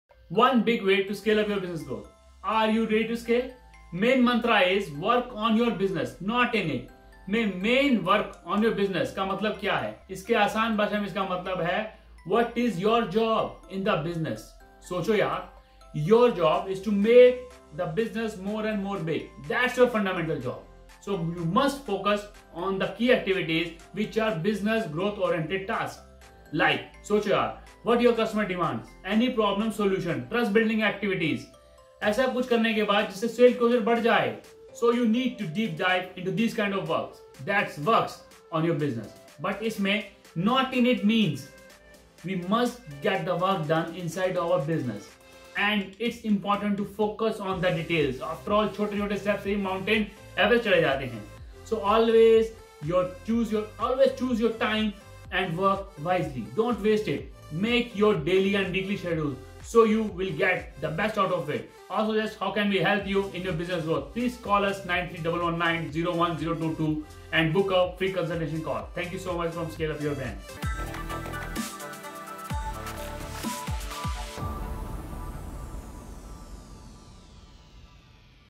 One big way to scale up your business growth? | Scale Up Your Brand
Big ways to scale up your business growth?

Are you ready to scale? So the main mantra is- Work on your Business, Not I it. in simple words this means What is your job in the business. Give it a thought, your job is to make the business more and more big. This is your fundamental job, so you must focus on the key activities which are business growth oriented tasks. Think about what your customers demand, any solution for your problems, trust building work anything that improves your sale closure.

So you need a deep dive into these kind of work. That work on your business and not in it. That means we must get the work done inside our business and it’s important to focus on the details. After all  it is well said that said that small steps helps you reach the heights.

Always choose your time wisely, do not waste it. Be productive by making your daily and weekly schedules, for getting the best out of it.

Thank you so much from scale up your brand.